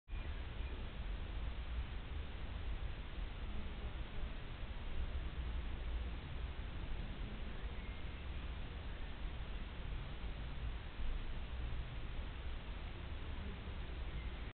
Amselnest Gefiederpflege
Auch Amselweibchen legen Wert auf ein geplegtes Äusseres.